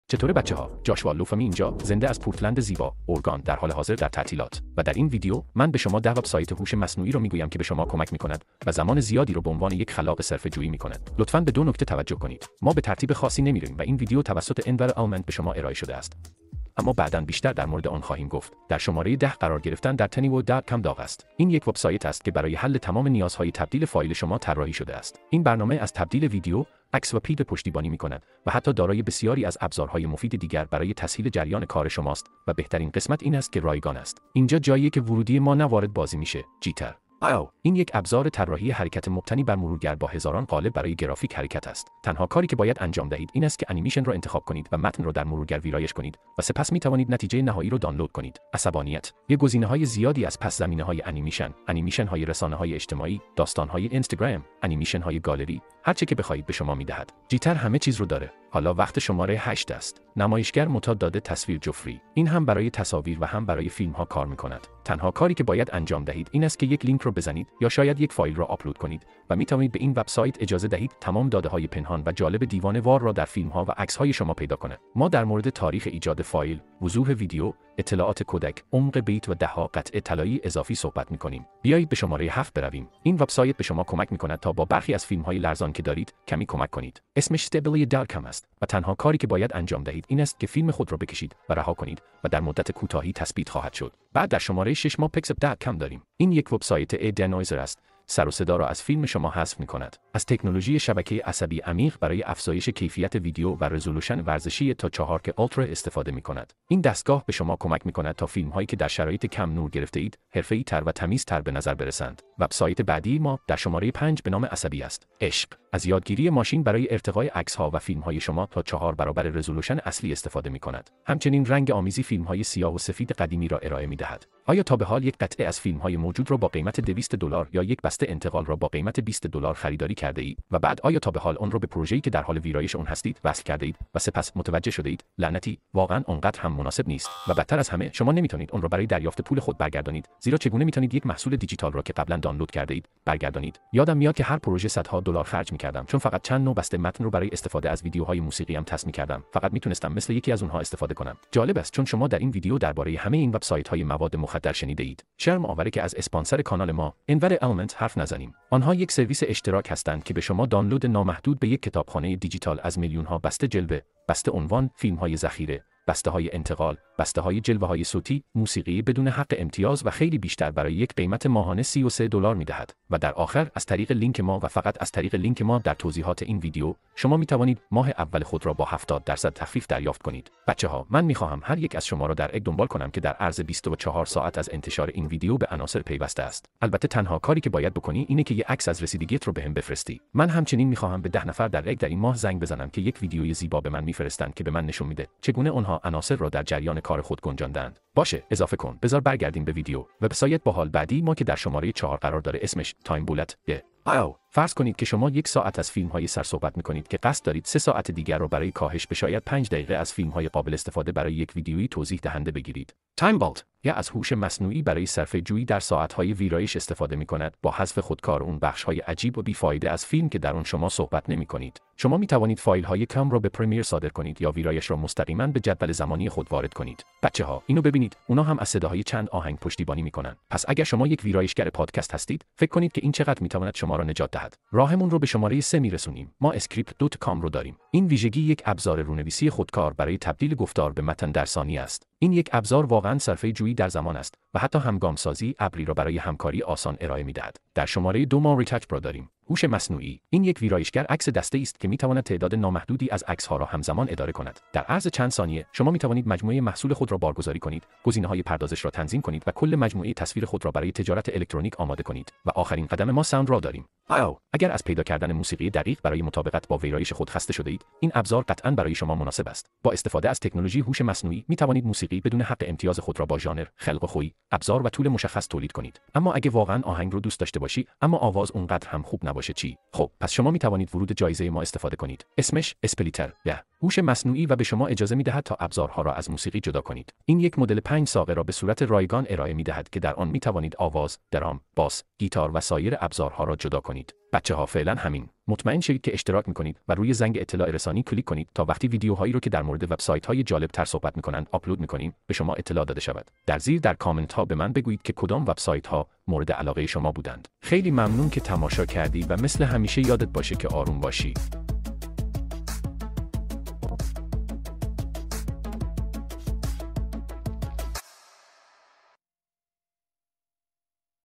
١٠ سایت که با هوش مصنوعی برای شما ویدئو حرفه‌ای تولید می‌کند - فارسی
با این ١٠ سایت حرفه‌ای ویدئو بساز

این ویدیو با هوش مصنوعی برای شما تولید شده است

دوبله فارسی